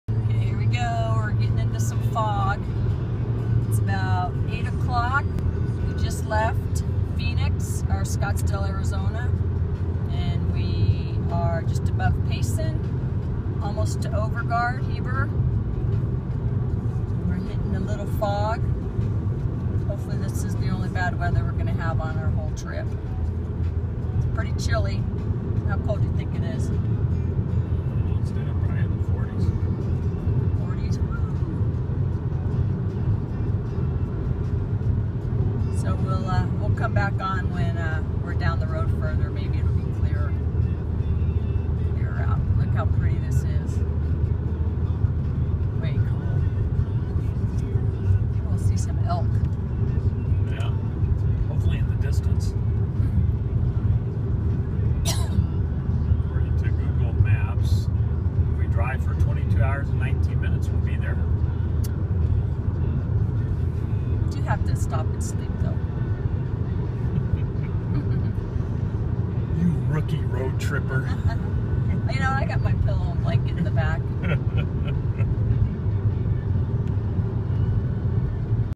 Fire truck adventure 5
Started our Adventure this morning at 6:00, 1100 miles to pick up the ladder truck.